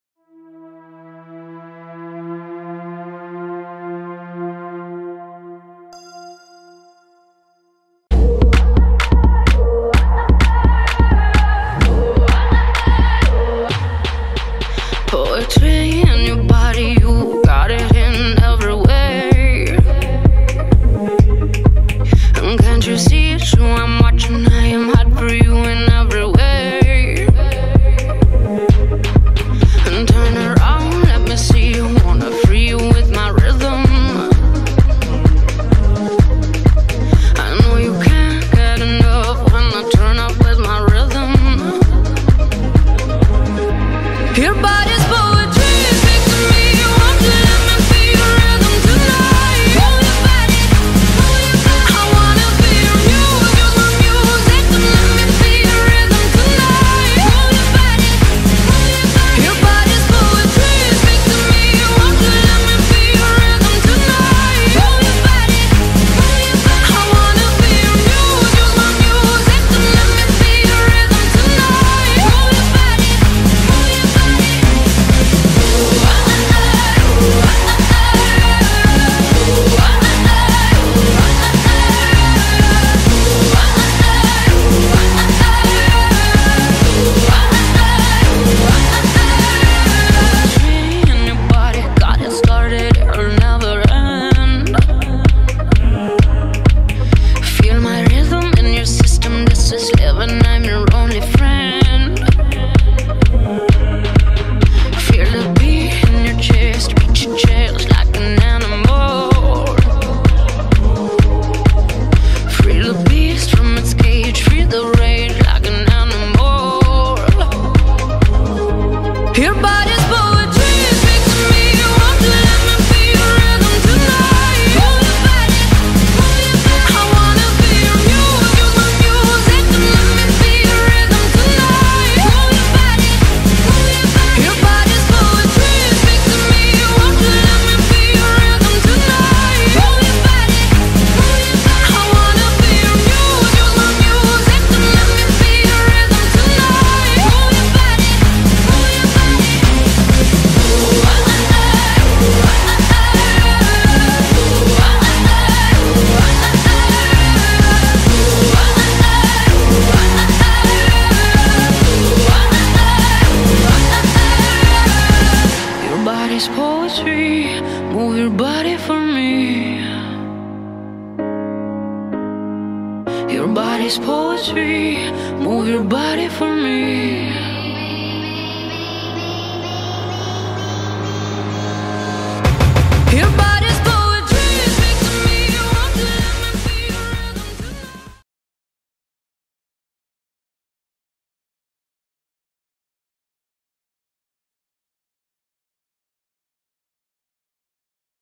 Stilt pole performer
Stilt pole performer with amazing costumes and movements that will wow mesmerize audience!